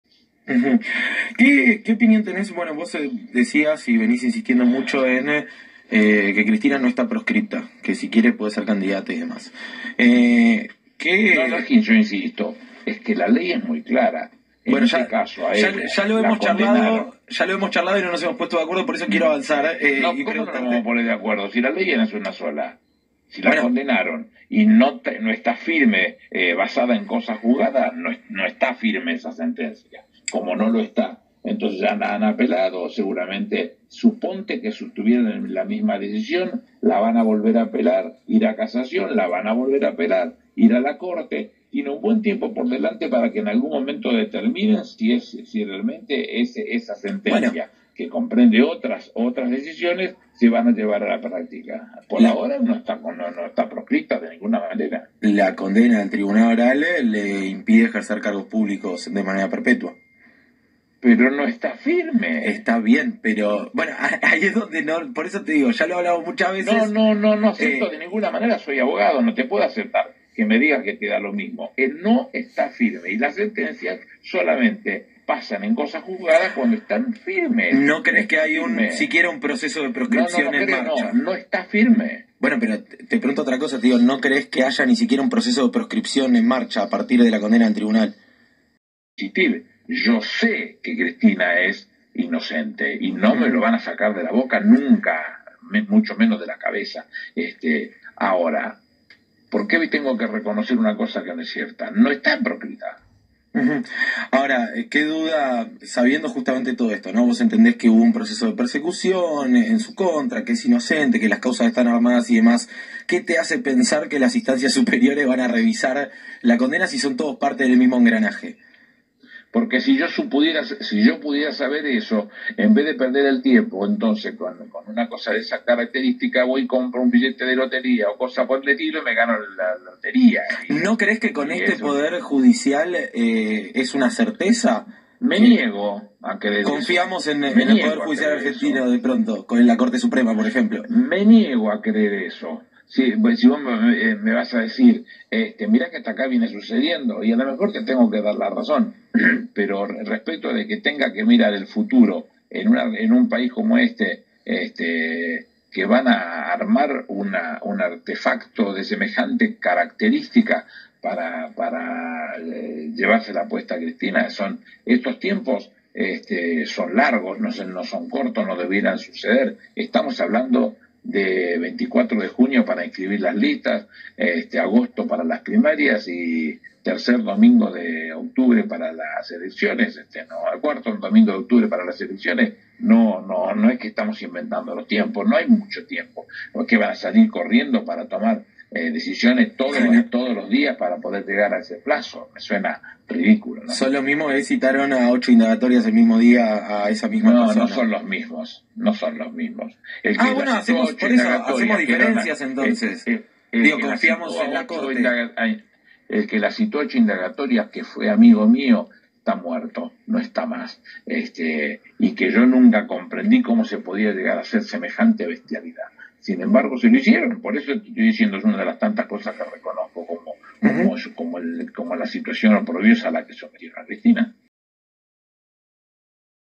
Anibal Fernández:" La ley es una y es clara ¡No hay condena firme y no hay proscripcion!"
El 25 de marzo de 2023 Anibal Fernández reafirmó qué Cristina no está proscrita, al no haber condena firme.
Sin embargo, el periodista ultrakirchnerista, no puede despegarse del discurso de odio hacia todas las instancias de apelación, los jueces y fiscales qué la condenaron y hacia la Corte Suprema.
La premisa es "la condena ya esta escrita", por lo tanto hay que deslegitimar al sistema judicial.
Anibal, no siendo santo de mi devoción, deja claro que no dan los tiempos jurídicos, para que se de la proscripcion.